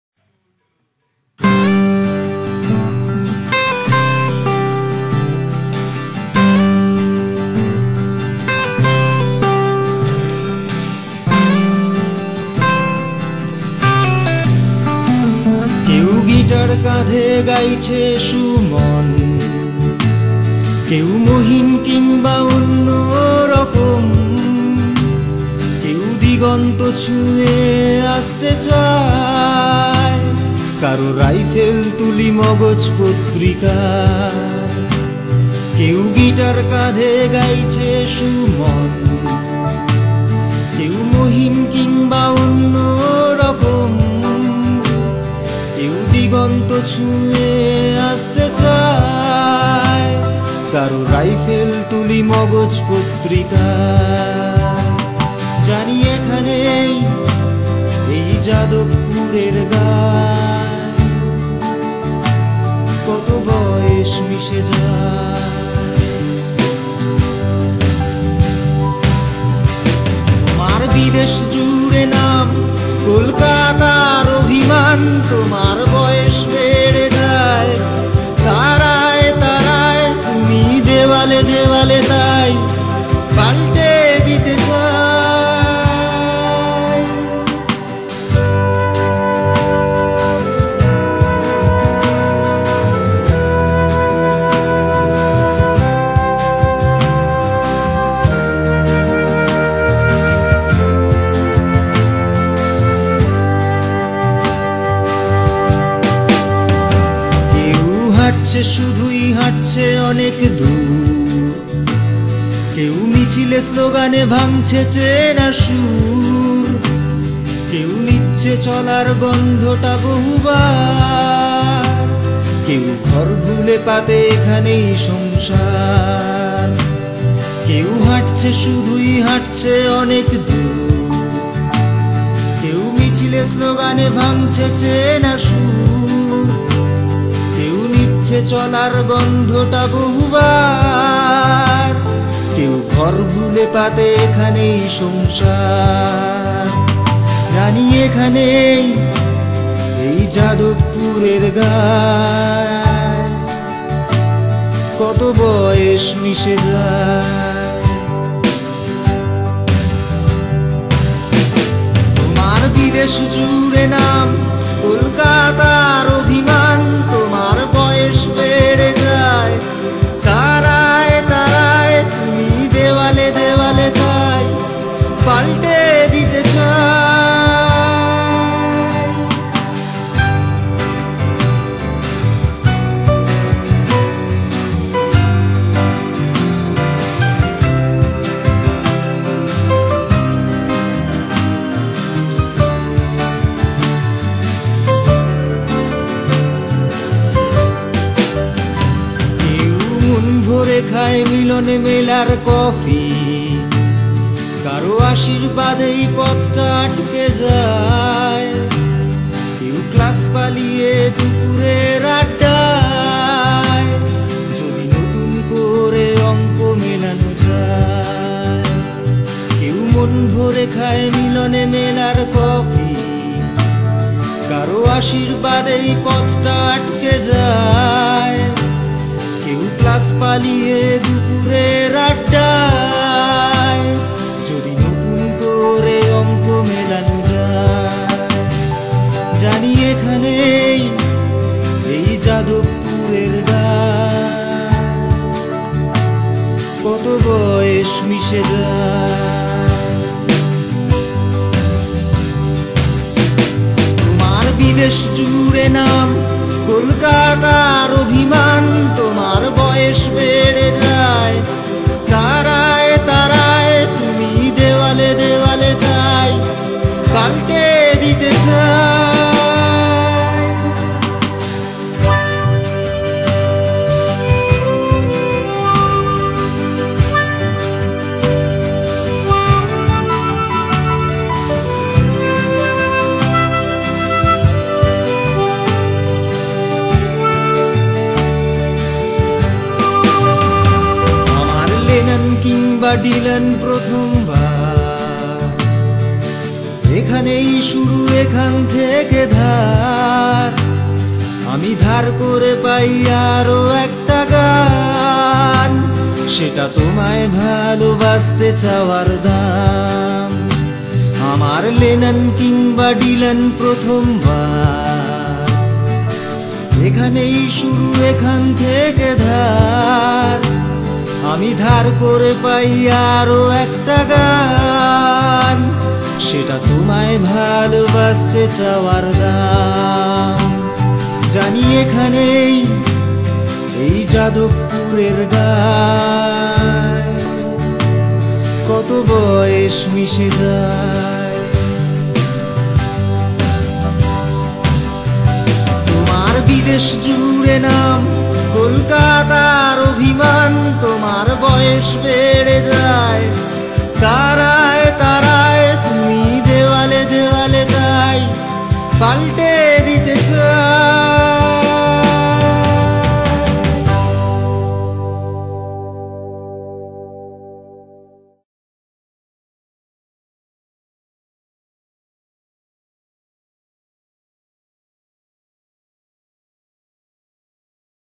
Jadavpur er gan.......
Song and documentary about JADAVPUR UNIVERSITY